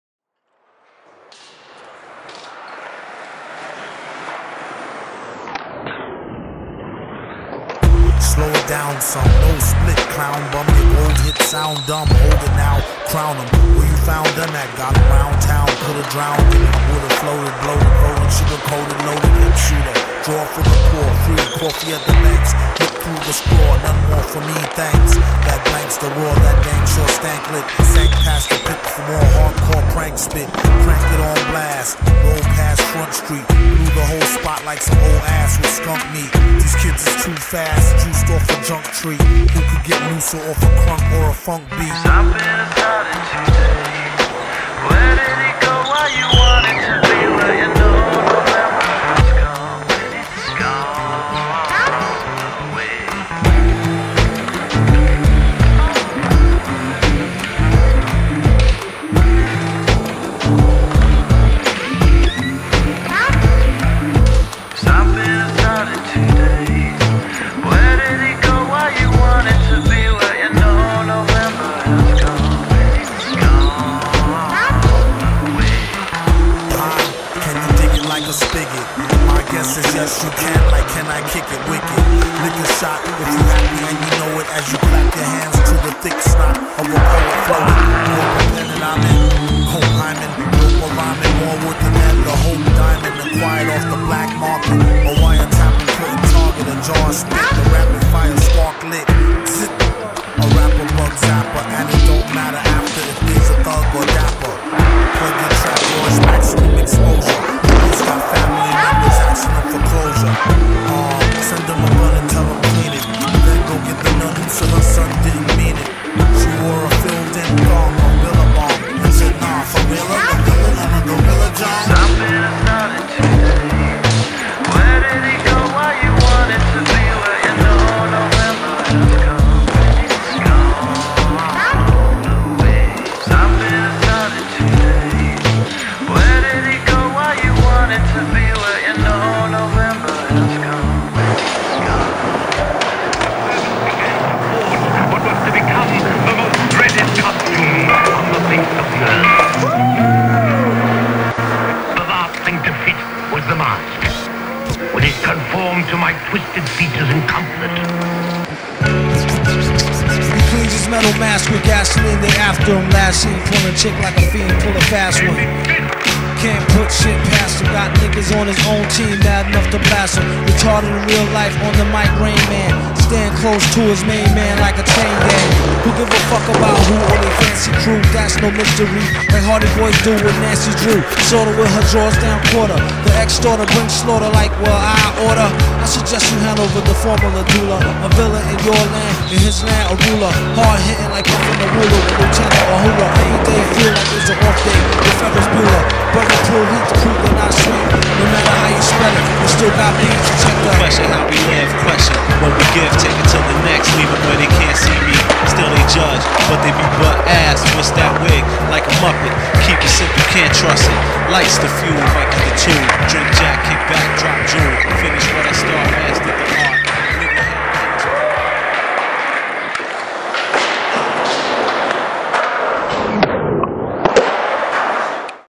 Home Movies Twenty Five | Indoor Life
First edit from the indoor here in Saskatoon.

Shout out to:
Jorell Clay
Josh Semchuk
Darcy Hannah
Brad Chovin
Nathan Gladue
Justin Schwan
Joel
Lynzi Bowser

Everyone at Ninetimes Skateshop